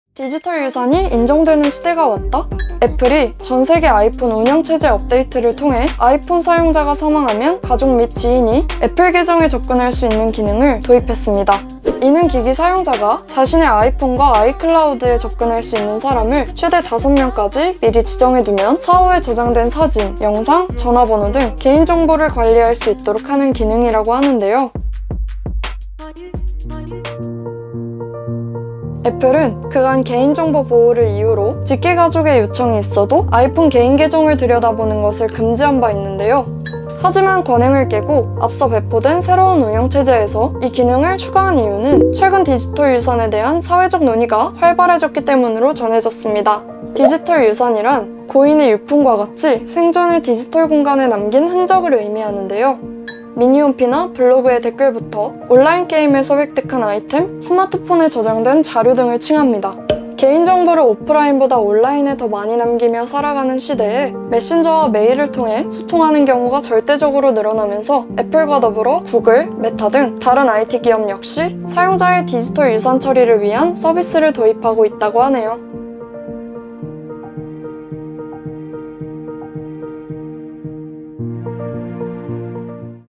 사용자가 사망해도 휴대폰 확인이 가능하다? 애플의 '디지털 유산' 기능
애플이 새로 배포된 iOS 15.2 버전에 ‘디지털 유산’ 기능을 새롭게 추가했습니다. 해당 기능은 친구나 가족이 사망하면 사전에 지정한 사람이 고인의 사진, 영상, 전화번호 등 중요 데이터에 접근할 수 있도록 하는 서비스인데요. 더 자세한 정보는 상단 영상에서 확인할 수 있습니다.